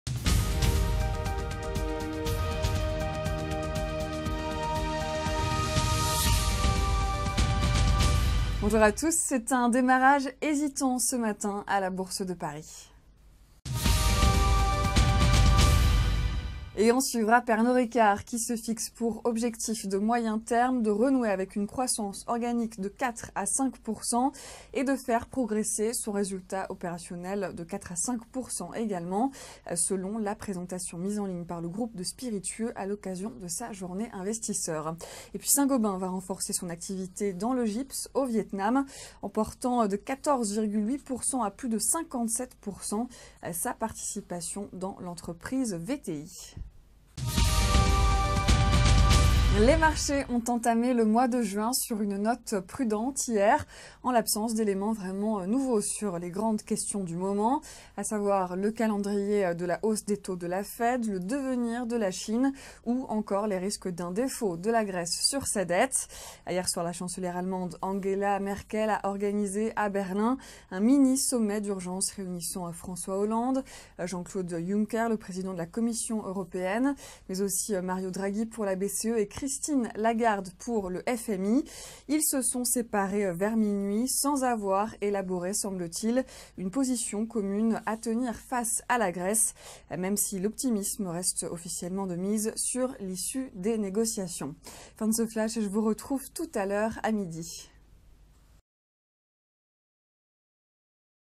02/06/2015 : Ouverture : prudence pour démarrer
Les marchés ont entamé le mois de juin sur une note prudente hier en l'absence d'éléments vraiment nouveaux sur les grandes questions du moment, à savoir le calendrier de hausse des taux de la Fed, ou les risques d'un défaut de la Grèce sur sa dette. La séance démarre dans l'hésitation à Paris, avec un CAC40 en hausse de 0,1%.